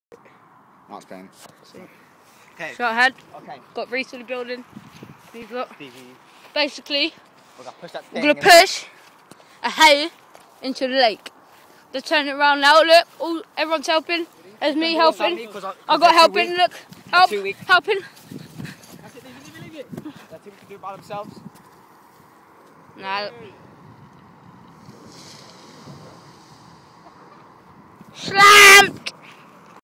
pushing hay in the lake
shahad (me) charlie,stevie and lewie are pushing hay in a lake recored by reece crotty